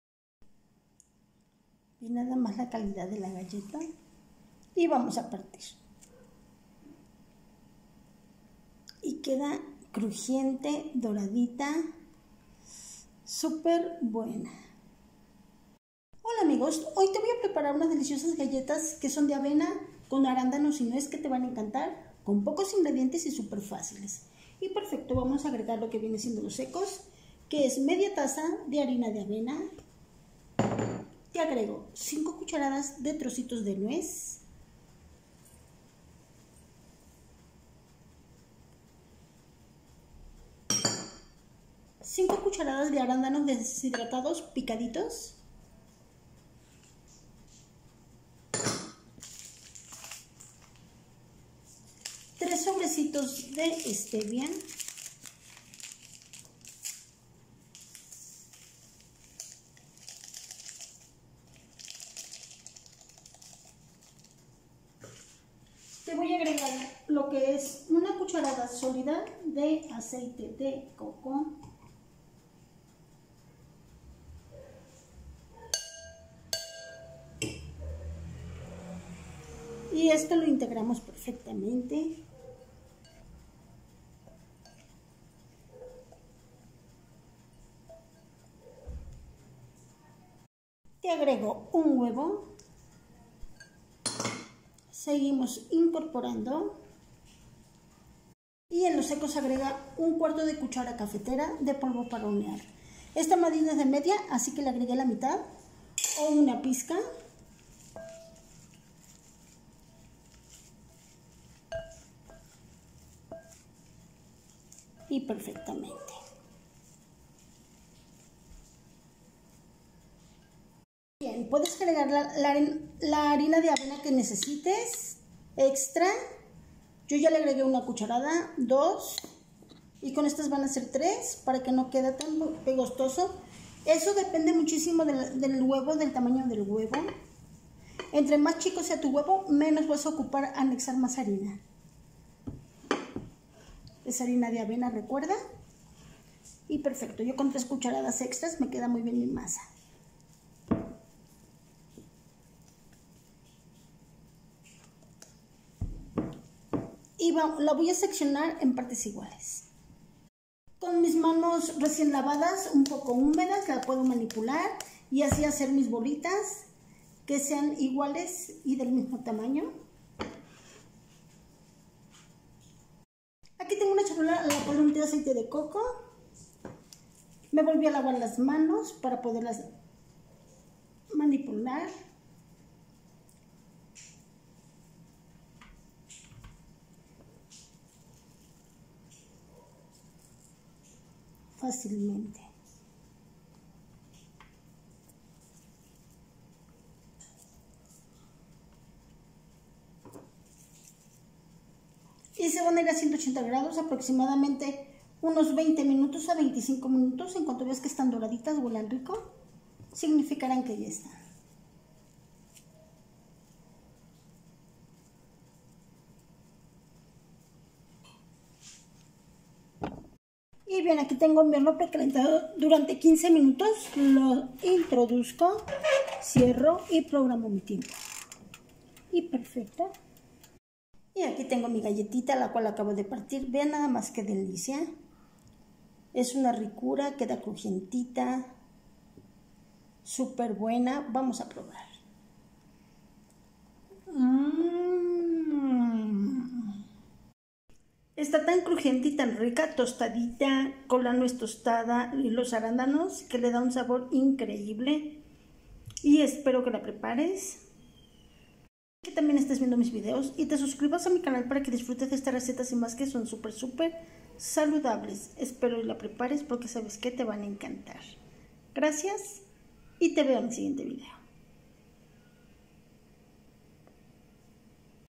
GALLETAS🍘DE AVENA ARÁNDANOS Y NUEZ
Crujientes y muy deliciosas estas galletas tan fáciles de preparar con pocos ingredientes y tan saludables... te invito a que las prepares🍘🌷🍘🌷🍘🌷🍘🌷🍘🌷

INGREDIENTES
1/2 taza de harina de avena
5 cuch. De trocitos de nuez
5 cuch. De arándanosdeshidratados y picados
3 sobresitos de estevia
1 cuch. Sólida de aceite de coco o 3 soperas
1 huevo
1/4 de cuch. Cafetera De polvo para hornear
3 cuch. De harina de avena

Para estar juntos en estas recetas súper súper saludables🌷